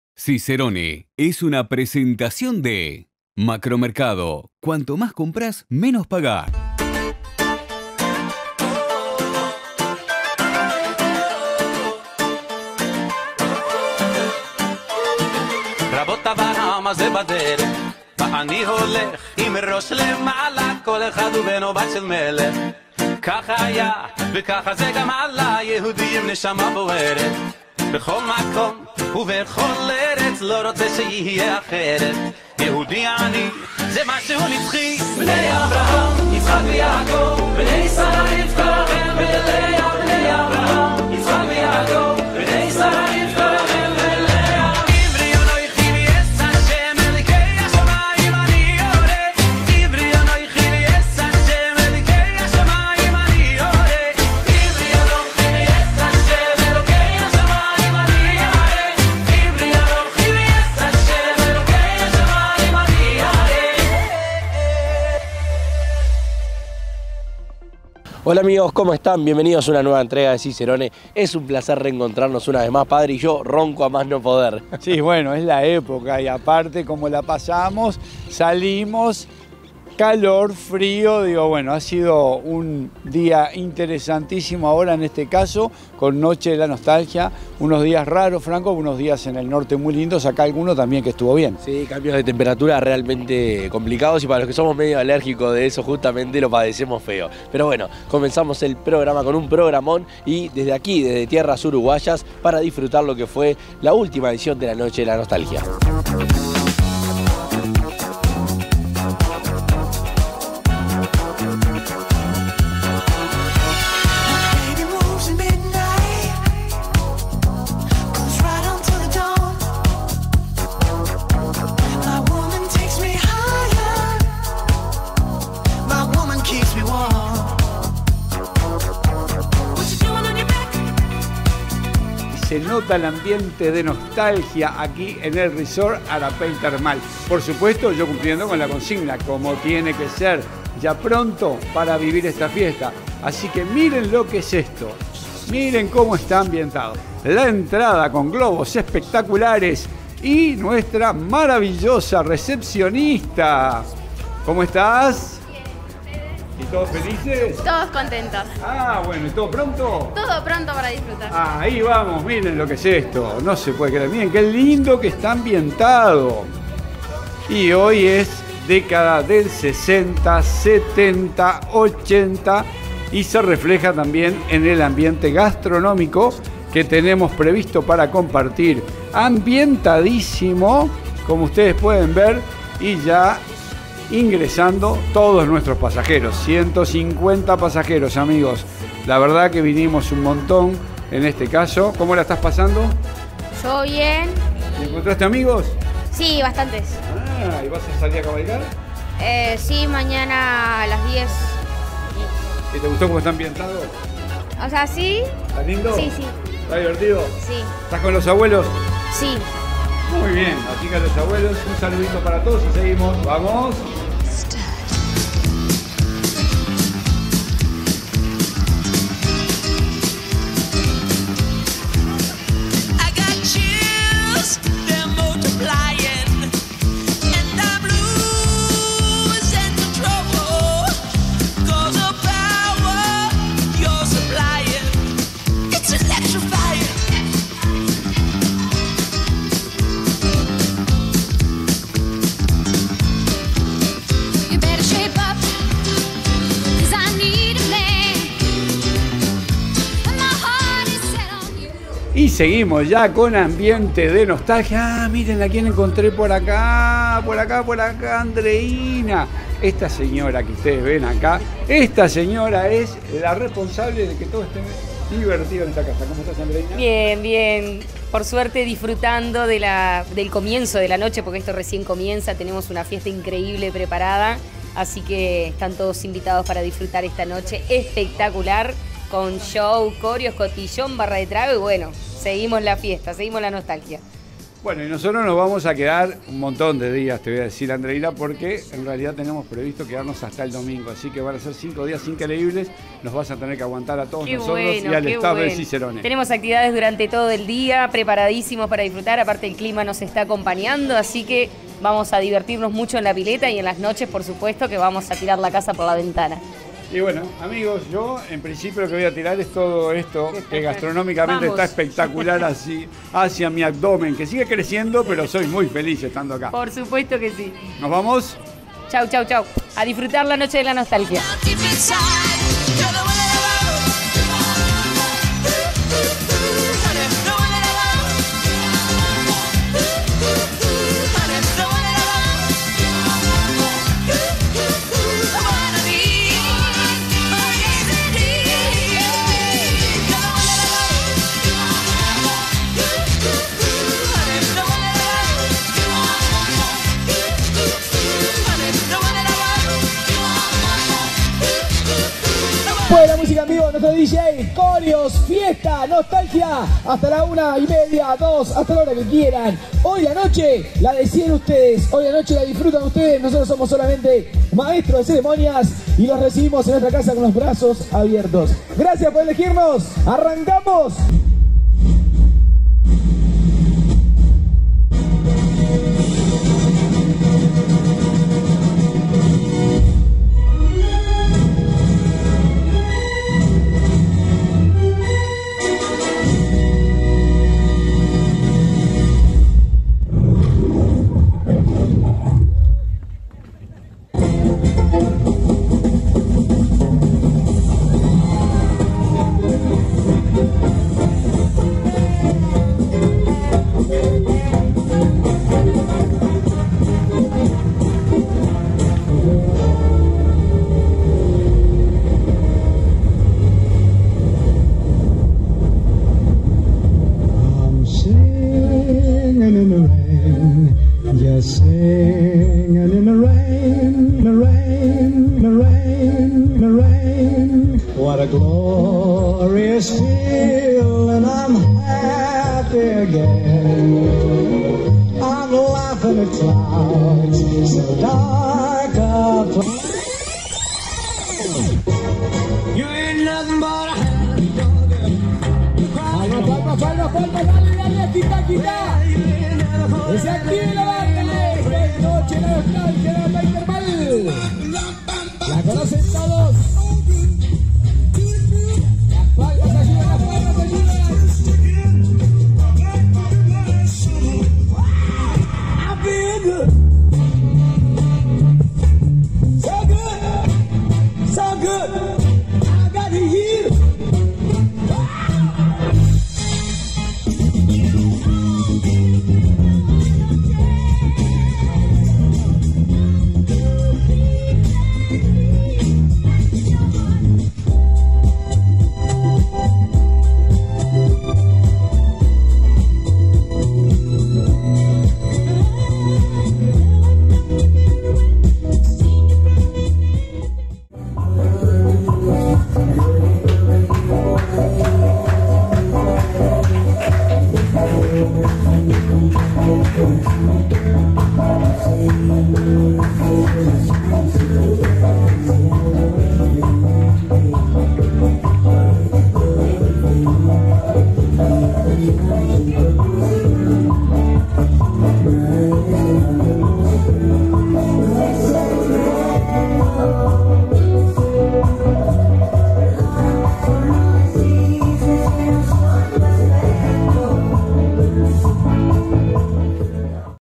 Nostalgia 2022 con Cicerone Latin Music en Arapey Thermal Resort and SPA!!
Como ya es una costumbre cada año hacemos una fiesta de Nostalgia inolvidable!!!

DTV Producciones
Club de Viajes Cicerone, Plaza Independencia 773, Hotel Radisson Victoria Plaza, Montevideo, Uruguay